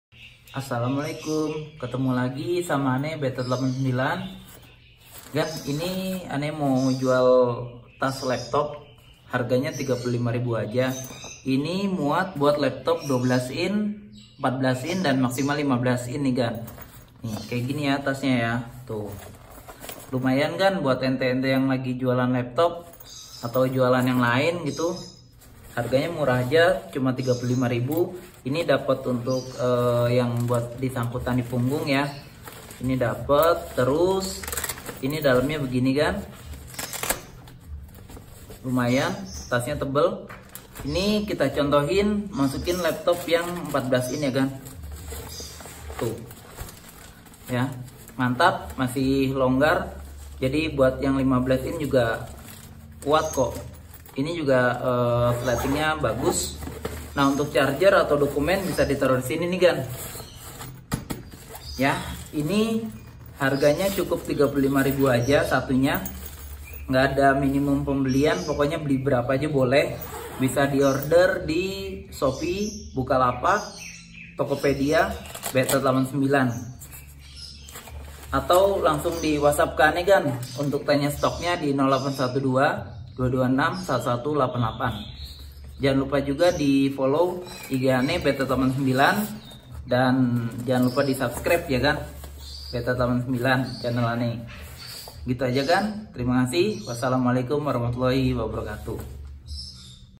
HOT SALE..!!! Tas Laptop Termurah 35k bisa untuk Maks 15” cuma di betet89
Bismillah
Assalamualaikum

Gan ane jual Tas Laptop murah ni harga cuma 35k/pcs
Tas ini yang biasa ane paketin dalam penjualan Laptop di Toko ane betet89

Monggo buat agan yang jualan Laptop atau mau di pakai sendiri atau di jual lagi juga bisa gan, langsung hubungi ane yaa gan

Atau bisa cek Tokopedia, Bukalapak, Shopee ane betet89

Terima Kasih gan
Wassalamualaikum